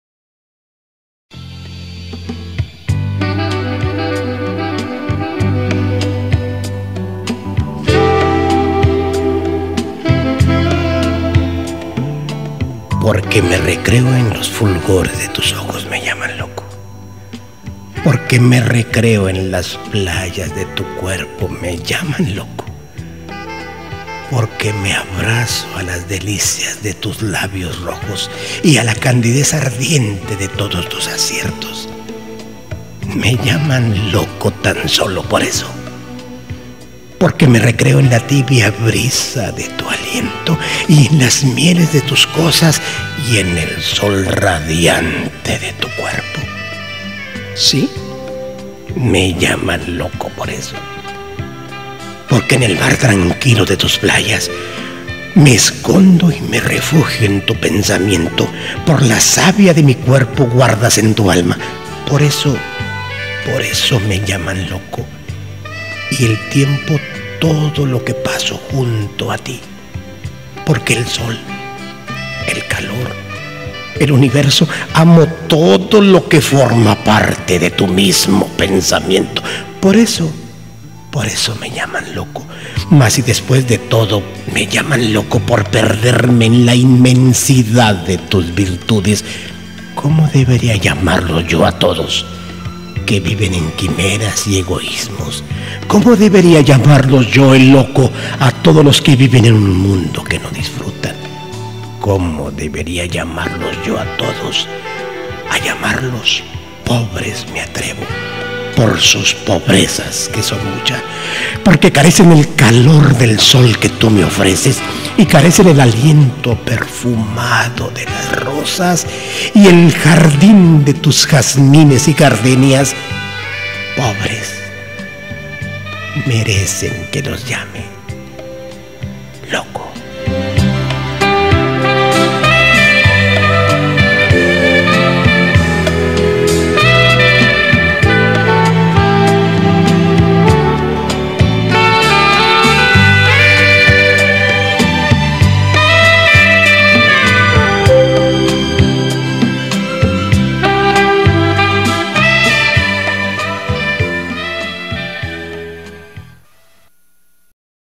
Poemas con Eduardo Javier ( Me LLaman loco )
Este poema es de la inspiracion del desaparecido amigo Maestro Manuel Martinez Valencia